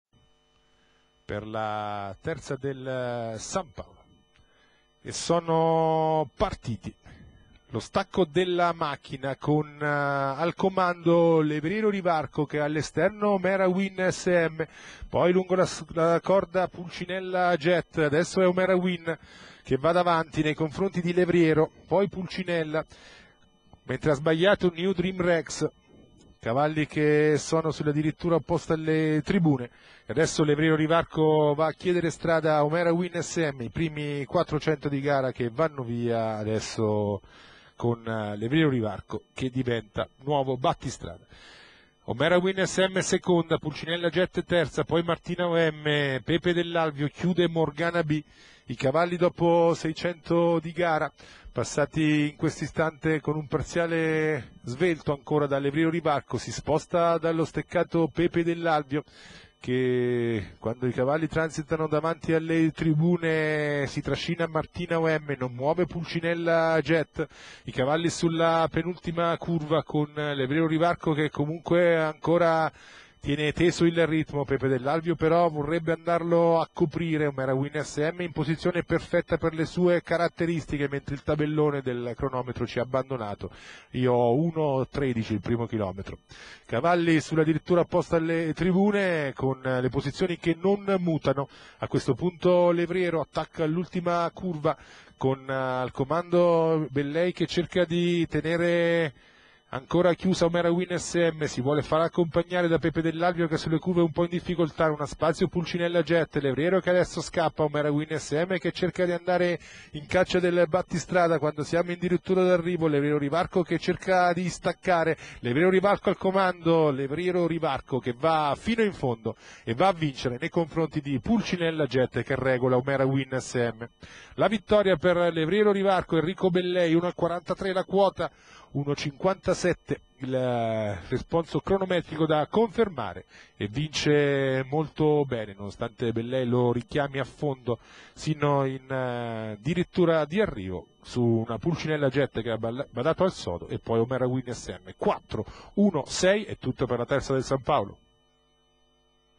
10/05/2014, Montegiorgio , Premio Mirtillo Rosso
1. LEVRIERO RIVARCO (BELLEI E.), 2. PULCINELLA JET (ANDREGHETTI R.), 3. OMERA WIN SM (DI NARDO G.), 4. PEPE DELL'ALVIO (CANGIANO D.), 5. MORGANA BI (MINNUCCI GP.), 6. MARTINA OM (BATTISTINI D.), 99. NEW DREAM REX (BASILE M.)